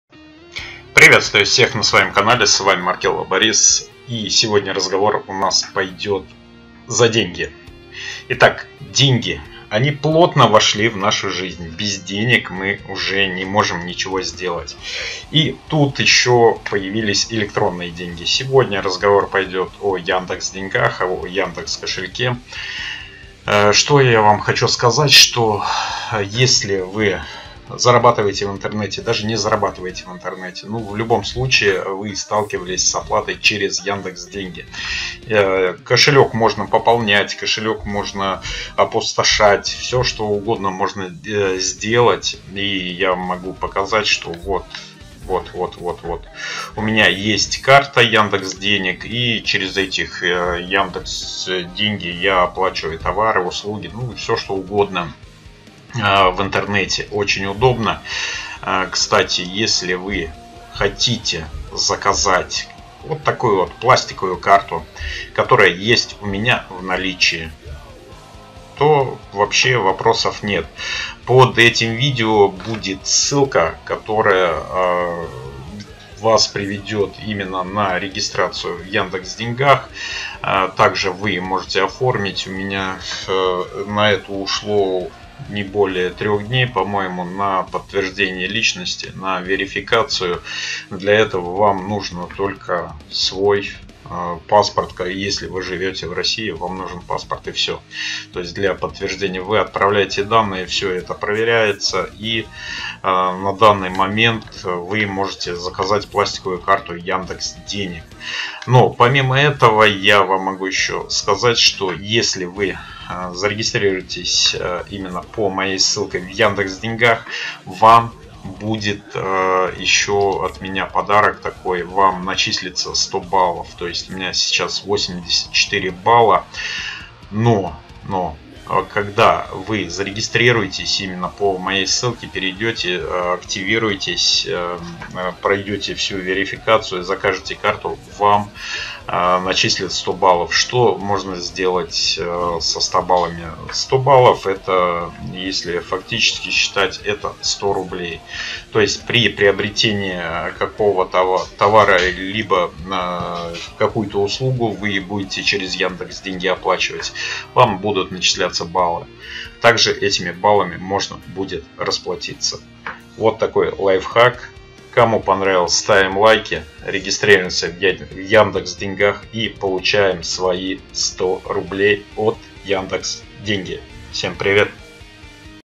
Деньги, Oчень Много Денег.Как получить 100 рублей в Яндекс Кошелек .СЕКРЕТЫ КОШЕЛЬКА ЯНДЕКС
Деньги, очень много денег Как получить 100 рублей в яндекс деньги СЕКРЕТЫ КОШЕЛЬКА ЯНДЕКС

Яндекс кошелек хранит большие секреты . Идентификация яндекс деньги дает большие возможности. Алиса поможет узнать yandex подробнее.